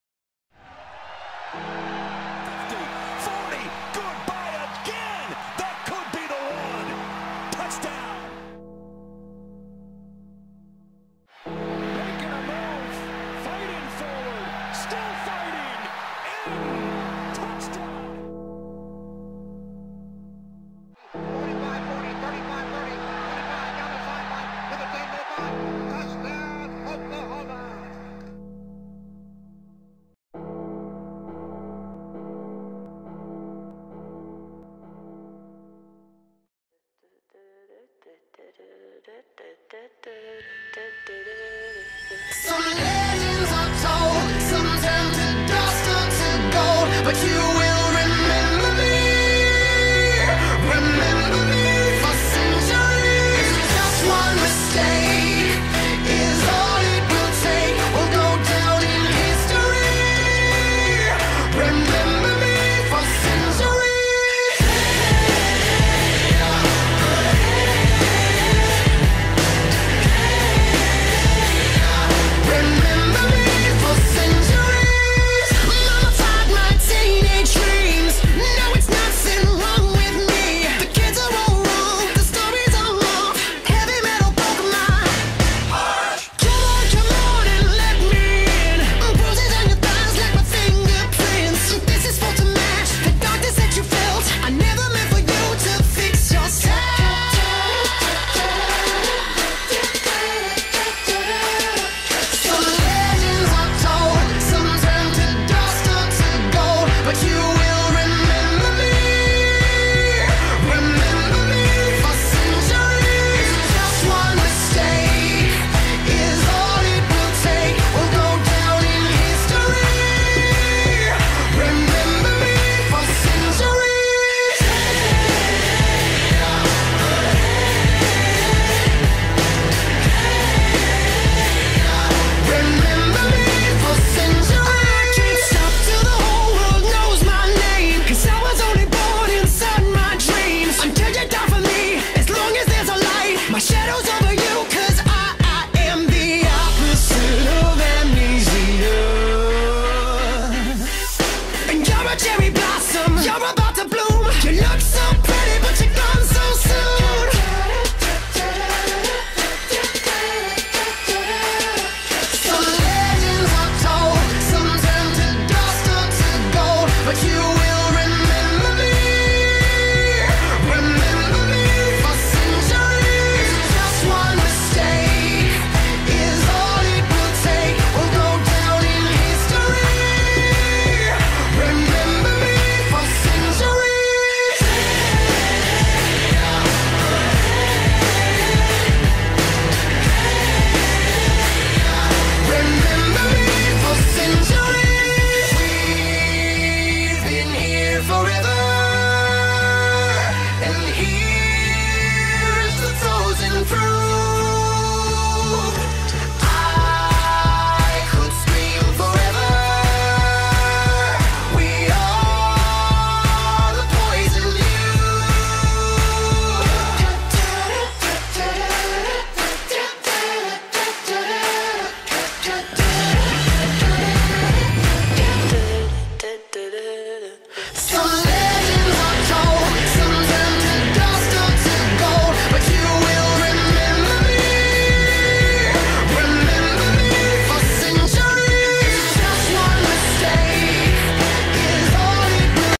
The Relentless Pursuit
Cassville Freshman football team continues to go after their goals of winning championships.